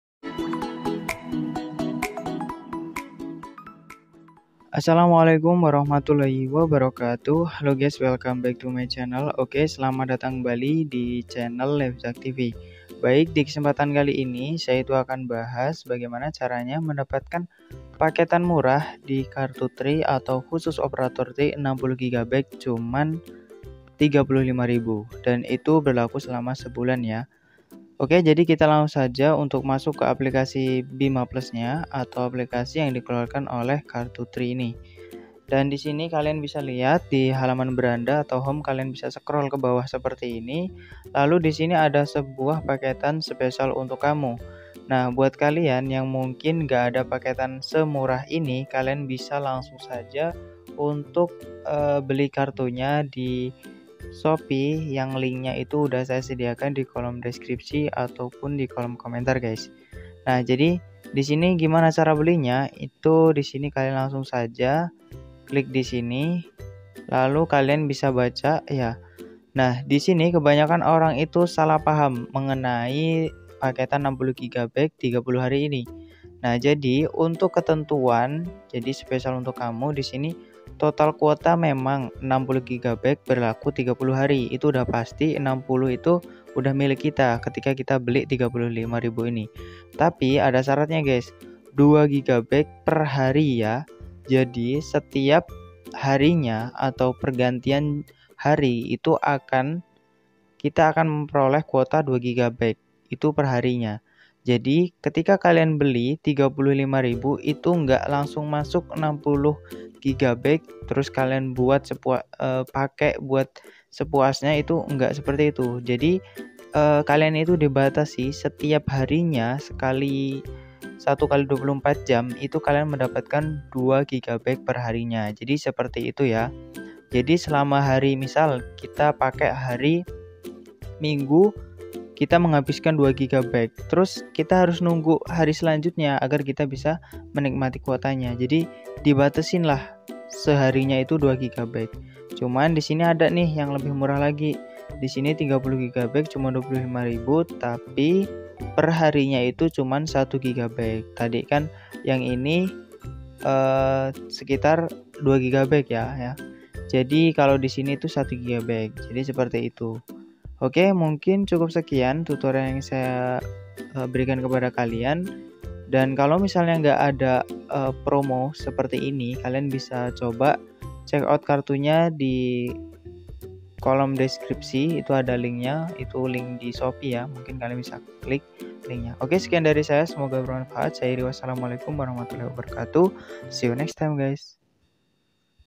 KUOTA Murah Tri...||...cara beli kuota murah di Bima plus RAHASIA BULAN DESEMBER
KUOTA Murah Tri...||...cara beli kuota murah di Bima plus RAHASIA

assalamualaikum warahmatullah wabarakatuh cara mendapatkan kuota murah Tri di aplikasi Bima plus untuk cara mendapatkan kuota murah ini kalian bisa simak video ini karena di dalam video ini saya menjelaskan cara mendapatkan kuota termurah di aplikasi Bima plus